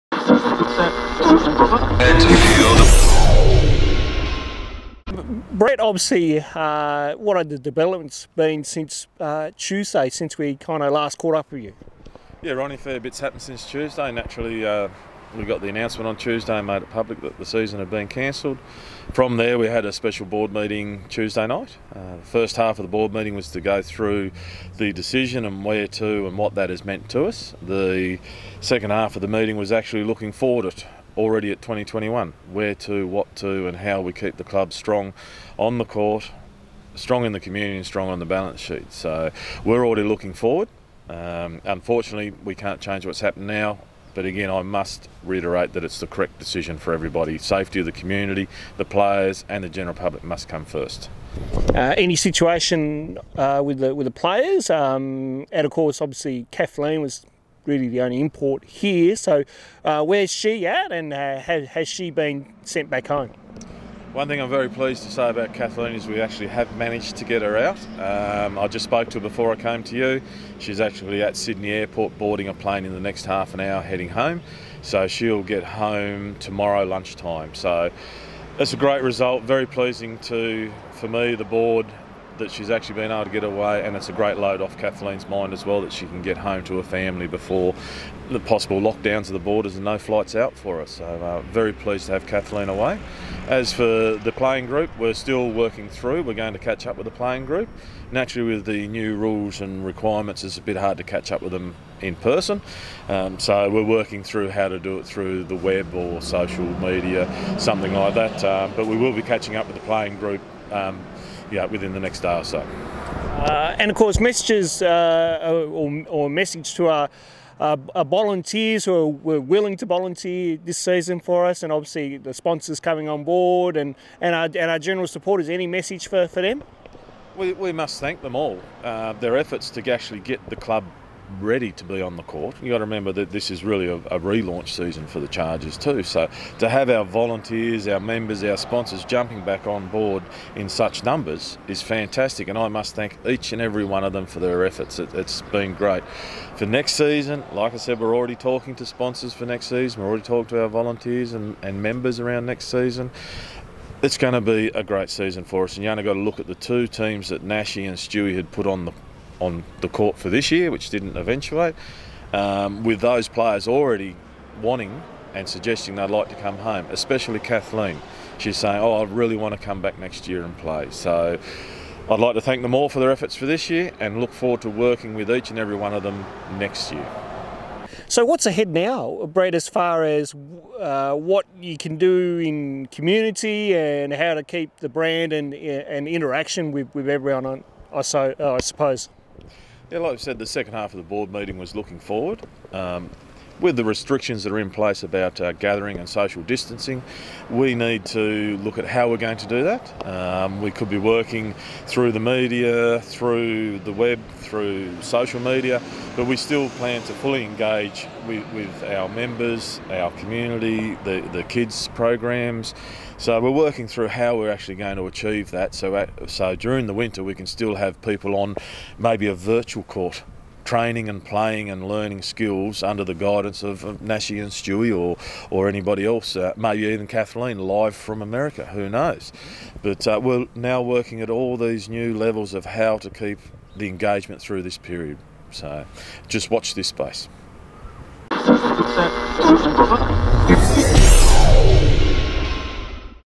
Chargers TV 27/3/20 Brett McKay
Hobart Chargers President Brett McKay with the latest update since Tuesday's cancellation of the 2020 NBL1 season.